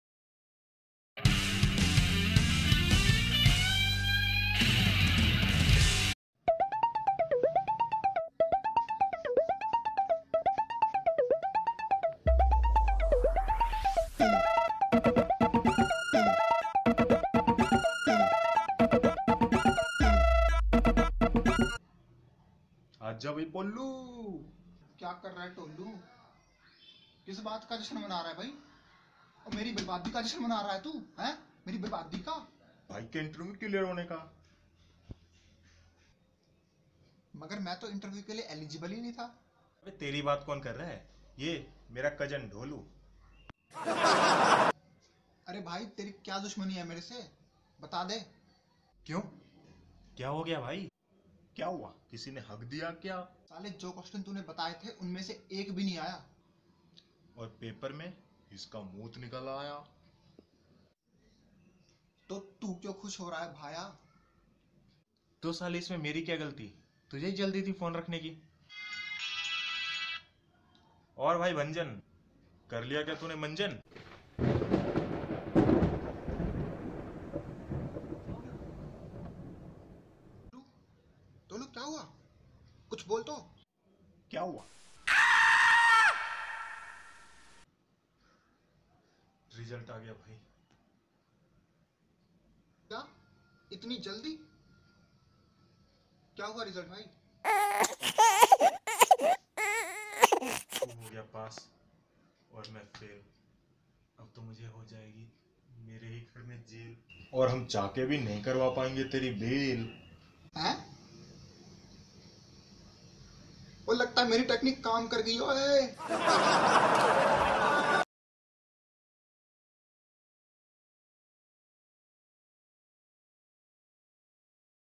Polu ka result || result hutiyapa || apm rocks - comedy
Tolu and polu result is declared. What are their results ? To know about their results watch this video.
This is an original work of Apm Rocks.
Watch, enjoy and share!!

Actors : Prashant Mishra, Nishant Kumar
Director: Apm Rocks